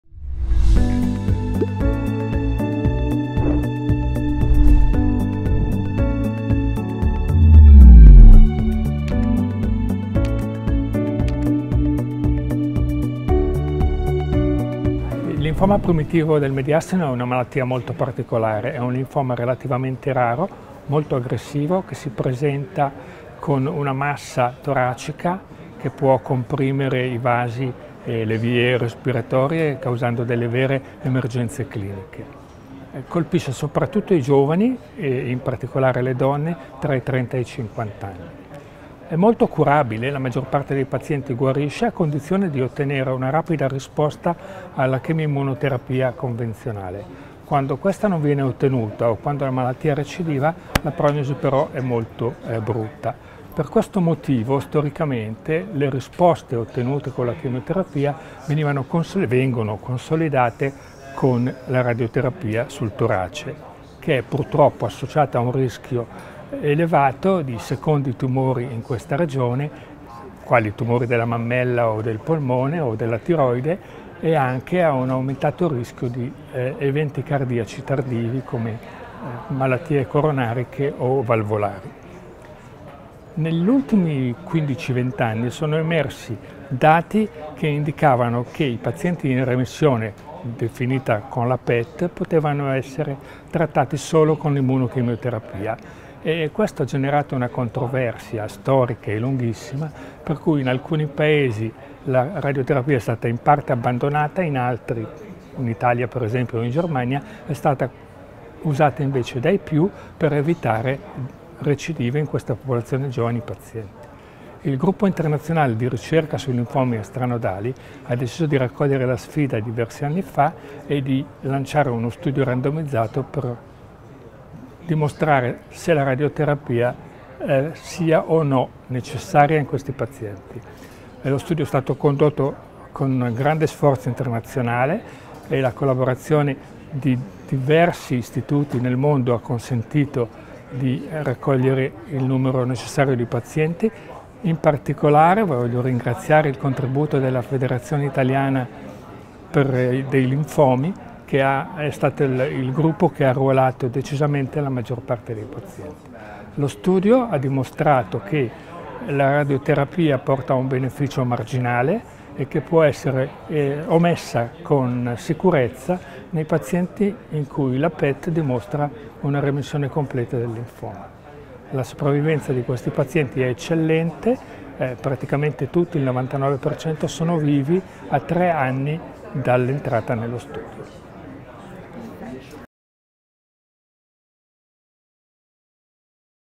PMBL, la radioterapia può essere omessa?
I risultati dello studio IELSG37 dimostrano che nel  linfoma primitivo del mediastino a grandi cellule B la radioterapia di consolidamento può essere omessa in sicurezza in pazienti con che abbiano ottenuto una remissione completa, verificata con PET, a seguito di immunochemioterapia. Le parole di Emanuele Zucca, Viceprimario Oncologia medica, Istituto Oncologico della Svizzera Italiana.